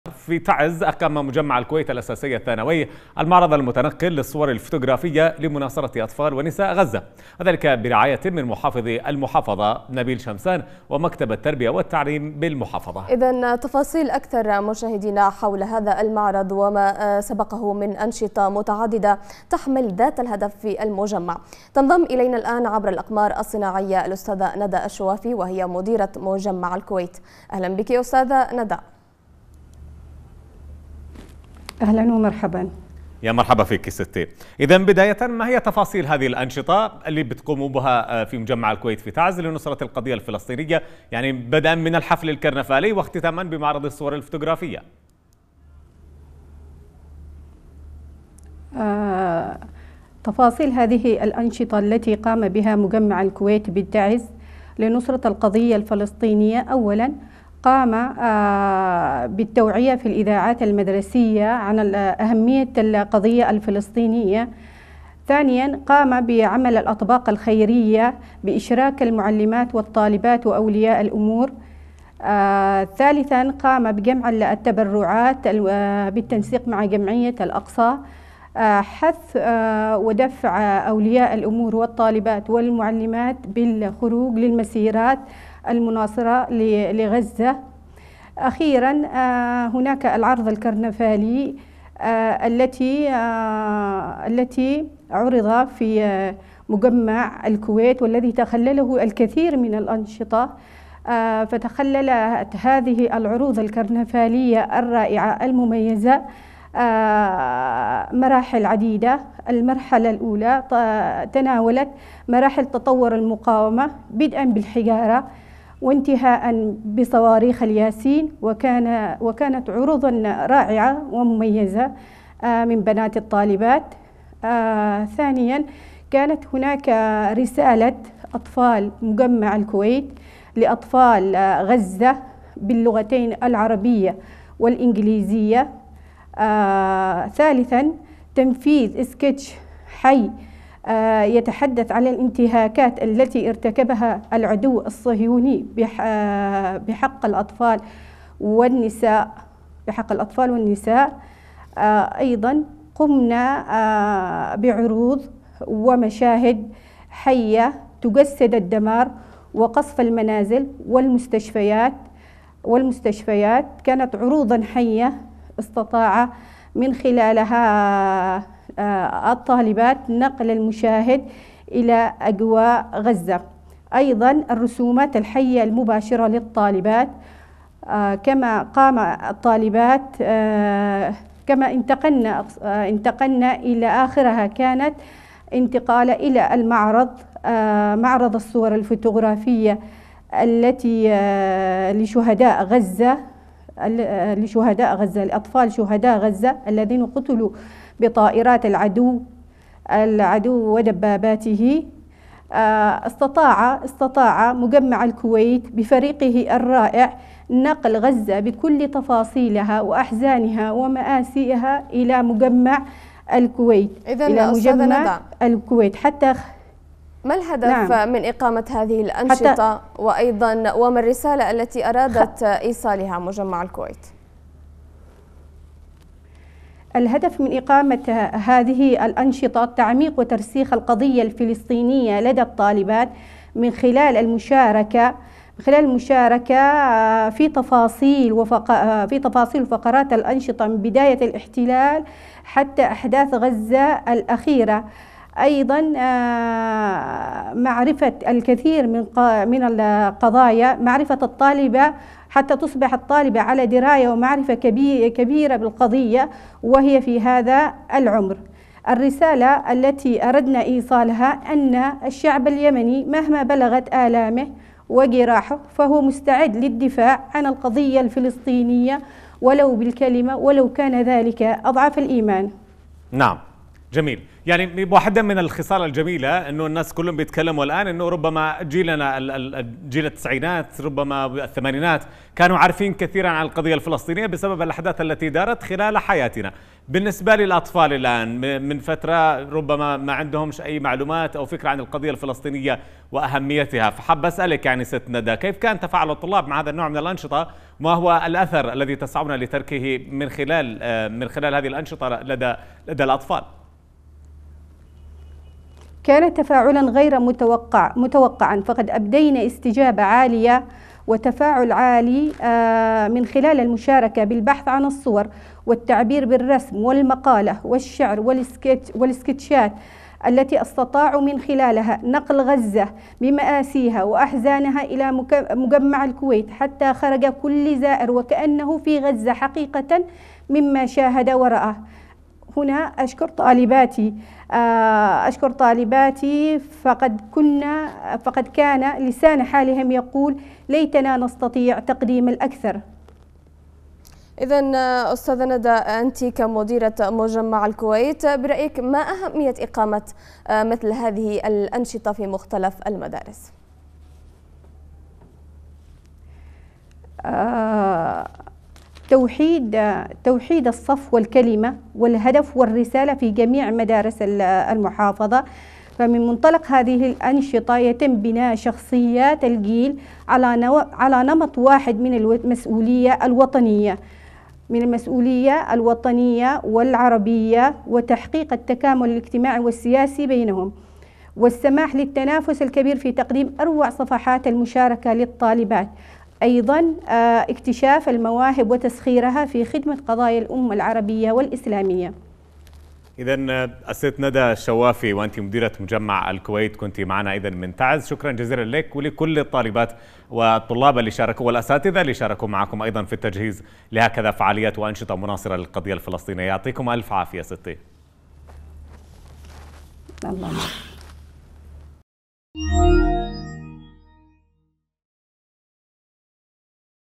معرض صور فوتوغرافية بمجمع الكويت بتعز لتوثيق مجـ.,ـازر الاحتـ.,ـلال في غـ.,ــزة
|  مع مديرة مجمع الكويت ندى الشوافي

قناة فضائية يمنية مستقلة تبث على مدار النايلسات بالترددات التالية :
HD     11257/H/27500
SD     11391/V/27500

صفحتنا المنوعة على الفيس بوك